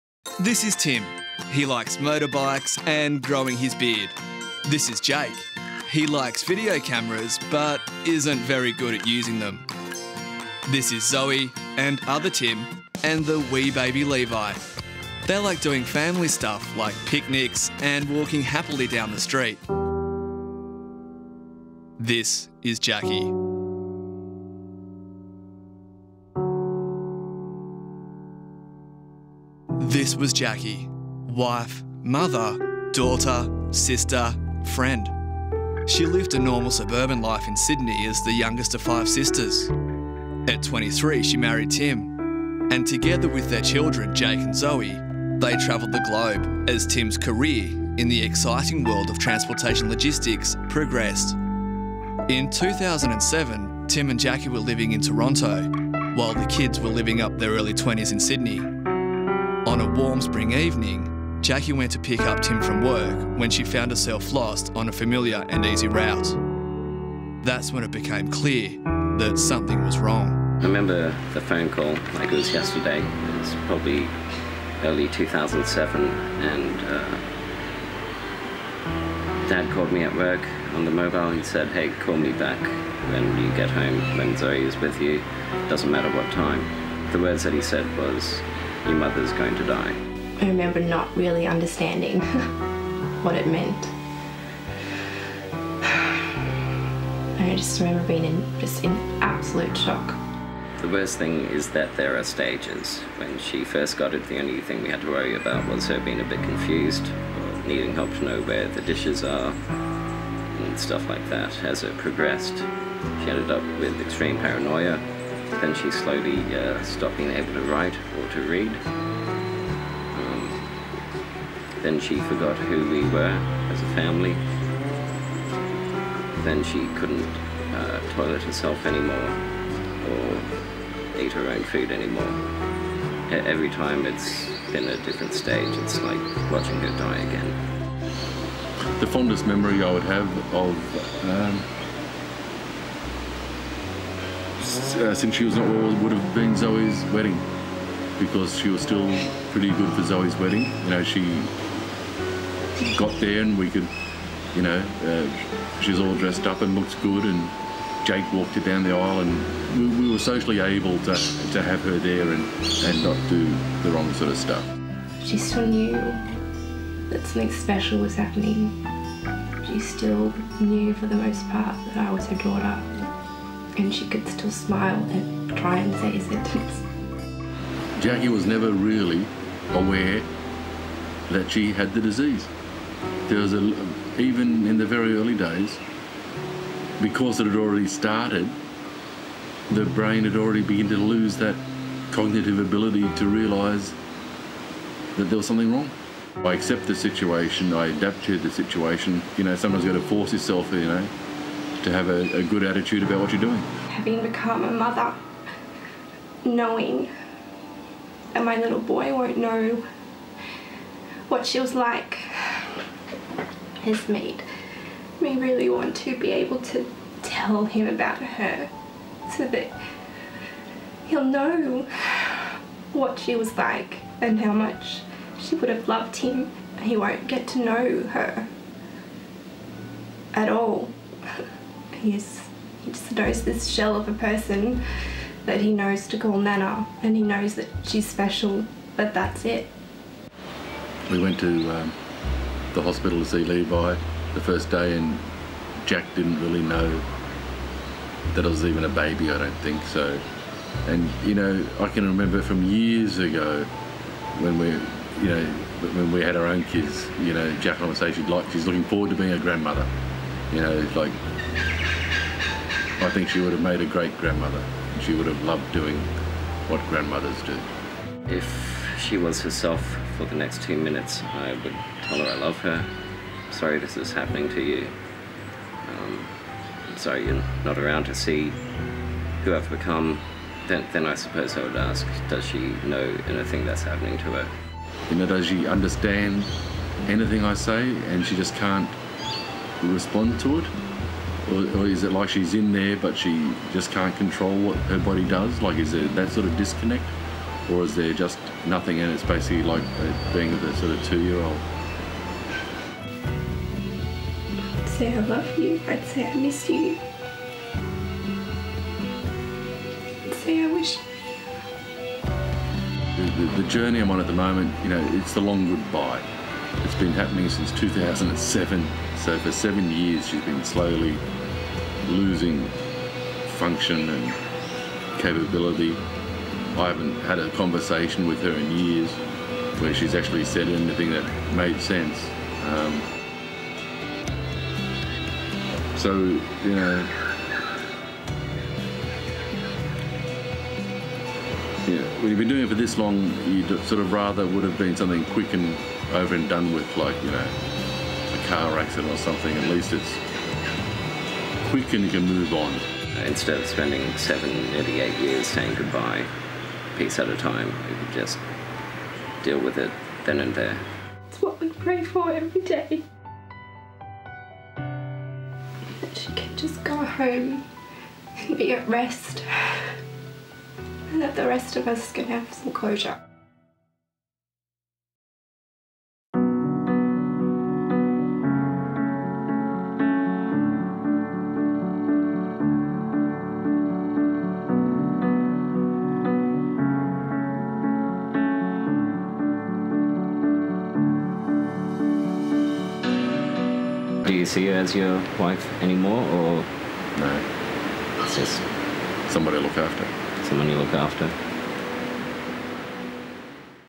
This Is Not Jacquie Anymore
This is not Jacquie anymore, but we still love her the way she loved us.

Jacquie passed away on the 17th of June, 2017 - 3 days before her 60th birthday.

or search on Facebook "Always Remember This book" to read for free.